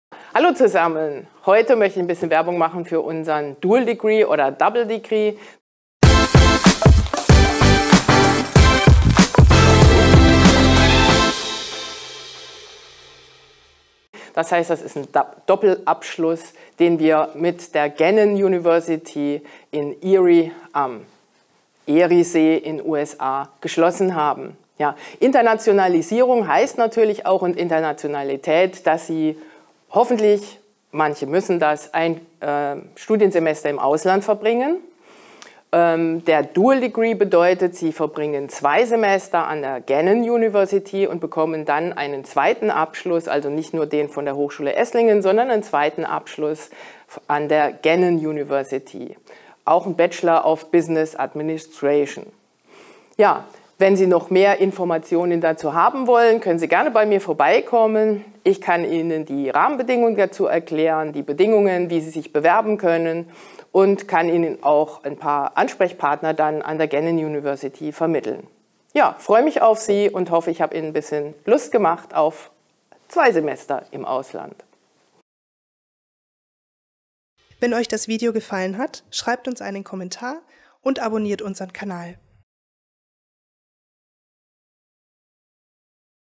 Double Degree - "Zwei Abschlüsse mit nur zwei Auslandssemestern" - Prof. Dr. Uta Mathis
Prof. Dr. Uta Mathis erklärt die Möglichkeiten des Double Degree, den unsere Studierenden in dem Studiengang Internationale Technische Betriebswirtschaft haben.
Ihr studiert bei uns in Esslingen, aber während eurer 7 Semester bei uns verbringt ihr 2 davon in Gannon, USA. Dadurch bekommt ihr nicht nur den Bachelor-Abschluss bei uns, sondern auch den Abschluss: Bachelor of Science in Business Administration in International Management (BSBA) der Gannon University. Damit seit ihr beim Berufseinstieg nochmal besser aufgestellt als eurer Mitbewerber um Jobs!
Und ein Auslandssemester ist für jeden persönlich eine tolle Erfahrung, an der man wachsen kann.